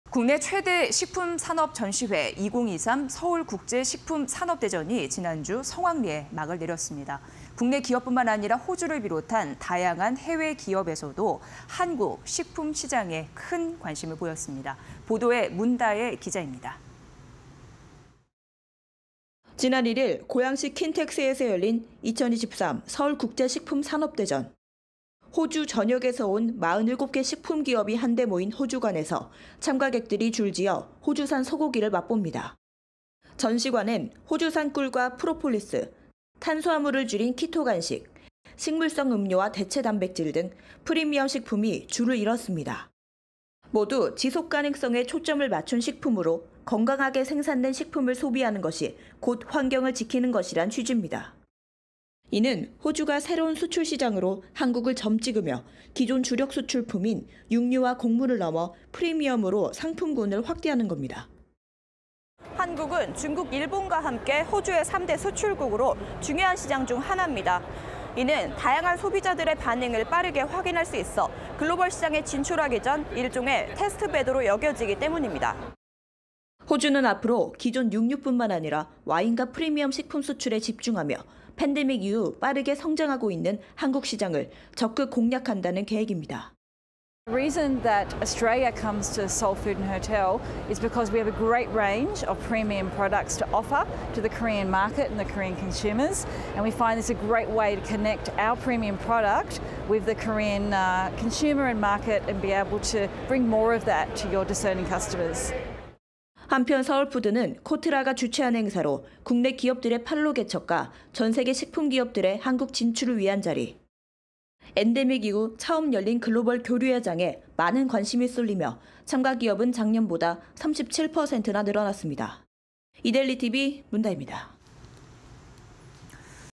캐서린 호주대사 "한국은 3대 수출국... 프리미엄 제품 확대" (20230605)
캐서린 호주대사 "한국은 3대 수출국... 프리미엄 제품 확대"